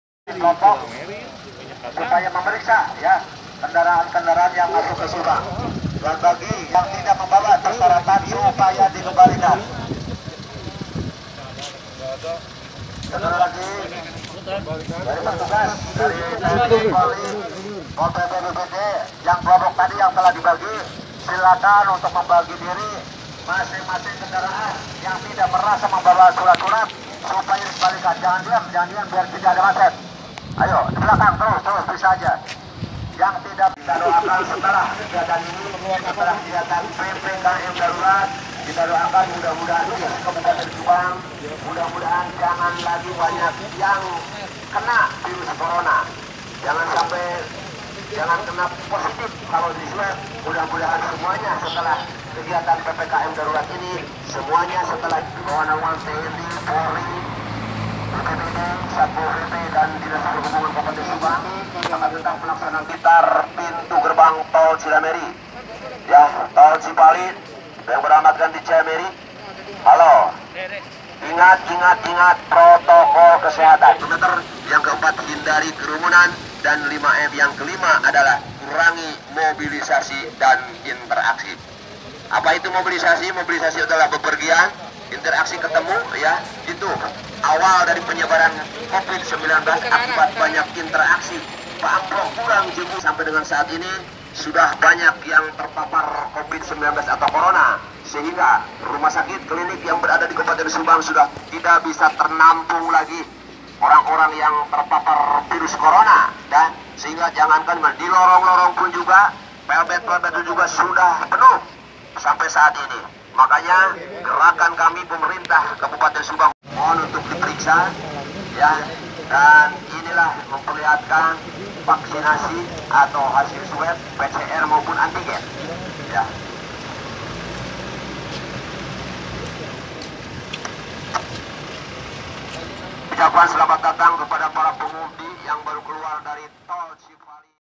PPKM Darurat, Ada Penyekatan di Tol Cipali Subang
Penyekatan di tol Cipali Subang.